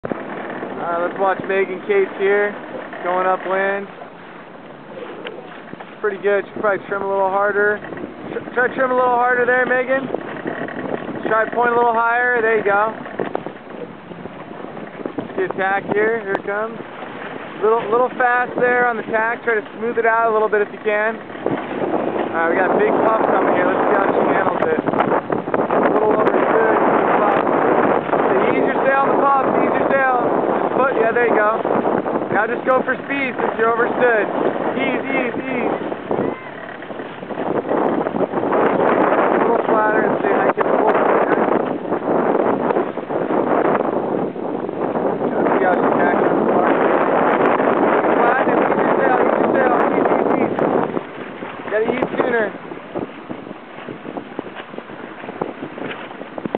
Short Course Sabot Sailing
Checkout this sailor as they sail upwind on this short course at practice.  She is very light but does a nice job keeping the boat flat and the leeboard in the water so that she doesn't slip sideways.  Also notice the tight outhaul, how she hikes the boat flat and looks around to see what her competition is doing.  Tactically she has sailed towards a big puff on the left and ends up sailing too far on starboard which ends up making her overstand (sails extra distance) the mark.  During her first tack she could improve by slowing down her turn very slightly.  On port, sailing to the mark, she should try to stay hiking and ease her sail to keep the boat flat.

When she reaches the mark she does a nice slow rudder turn, but struggles to get to a hiking position right away.  This is a tough maneuver in a puff, but you can do it! Coming out of the tack it is important to get hiking immediately since your weight on the rail will help the boat turn down.  Most beginners move into the boat a few seconds too early around the top mark.  The sail also needs to be eased very quickly to allow the boat to turn downwind toward your next mark.  In the perfect top mark rounding the weight on the rail and sail being eased cause the boat to head downwind with the rudder following the boat (fast) as opposed to forcing the turn (slow).